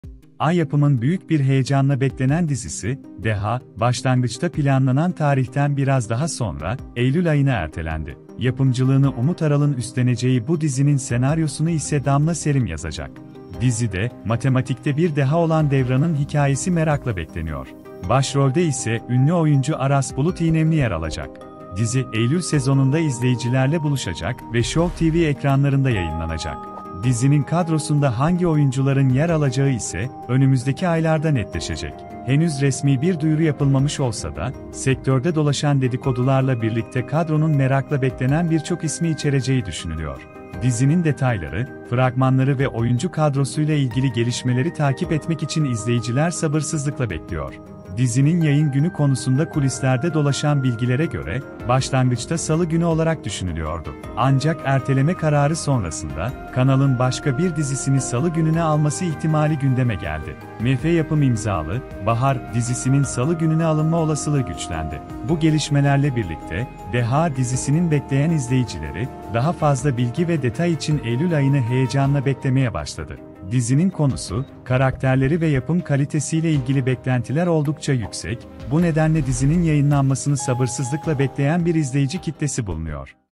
Deha Tanıtım | Ertelendi!
Ay Yapım'ın büyük bir heyecanla beklenen dizisi "Deha", başlangıçta planlanan tarihten biraz daha sonra, Eylül ayına ertelendi. Yapımcılığını Umut Aral'ın üstleneceği bu dizinin senaryosunu ise Damla Serim yazacak. Dizide, matematikte bir deha olan Devran'ın hikayesi merakla bekleniyor. Başrolde ise ünlü oyuncu Aras Bulut İynemli yer alacak. Dizi, Eylül sezonunda izleyicilerle buluşacak ve Show TV ekranlarında yayınlanacak.

Dizinin kadrosunda hangi oyuncuların yer alacağı ise önümüzdeki aylarda netleşecek. Henüz resmi bir duyuru yapılmamış olsa da, sektörde dolaşan dedikodularla birlikte kadronun merakla beklenen birçok ismi içereceği düşünülüyor. Dizinin detayları, fragmanları ve oyuncu kadrosuyla ilgili gelişmeleri takip etmek için izleyiciler sabırsızlıkla bekliyor.

Dizinin yayın günü konusunda kulislerde dolaşan bilgilere göre, başlangıçta salı günü olarak düşünülüyordu. Ancak erteleme kararı sonrasında, kanalın başka bir dizisini salı gününe alması ihtimali gündeme geldi. MF Yapım imzalı "Bahar" dizisinin salı gününe alınma olasılığı güçlendi.

Bu gelişmelerle birlikte, "Deha" dizisinin bekleyen izleyicileri, daha fazla bilgi ve detay için Eylül ayını heyecanla beklemeye başladı. Dizinin konusu, karakterleri ve yapım kalitesiyle ilgili beklentiler oldukça yüksek, bu nedenle dizinin yayınlanmasını sabırsızlıkla bekleyen bir izleyici kitlesi bulunuyor.